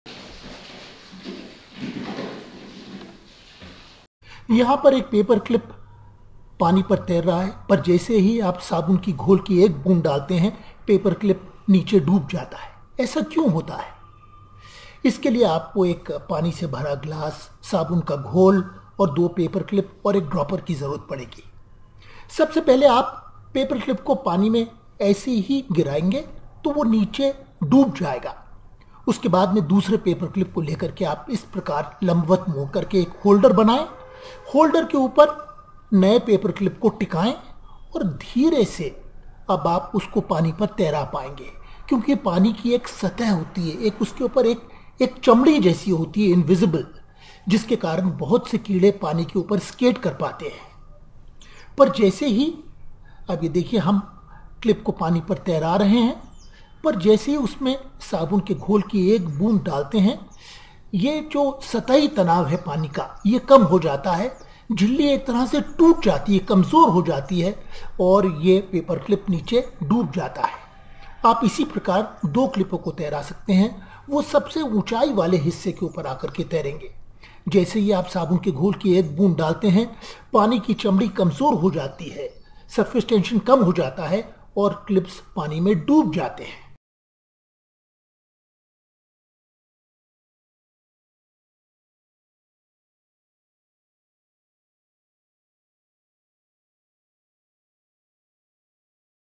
Floating Paper Clips | Hindi
Gently float a paper clip on water. On placing a drop on soap solution the clip will sink in the glass. Why does this happen? For doing this experiment you will need a glass of tap water, two paper clips, soap solution and a dropper. First try dropping a paper clip in water. The clip will sink to the bottom. Then open the second clip at right angles to make an improvised holder. Lay the clip on this holder and then gently float it on the water. Water has an invisible skin which enables some insects to skate on it. Now place a drop of soap solution near the floating clip. The soap solution will lower the surface tension of water – or weaken its skin and the clip will sink below.
 Try floating two paper clips – both will rise to the highest water level. On placing a drop of soap solution near the clips the water skin will weaken and both clips will sink to the bottom of the glass.